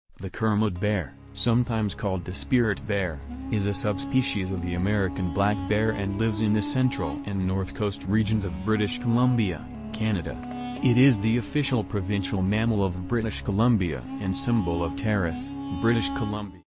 Kermode bear (aka spirit bear) eviscerates a salmon
The Kermode bear, sometimes called the spirit bear, is a subspecies of the American black bear and lives in the Central and North Coast regions of British Columbia, Canada. It is the official provincial mammal of British Columbia and symbol of Terrace, British Columbia.

source:
1. outdoorskieuncut "Instagram"  (Via @dutch_wildlife_photographer)

voiceover - t2s.ai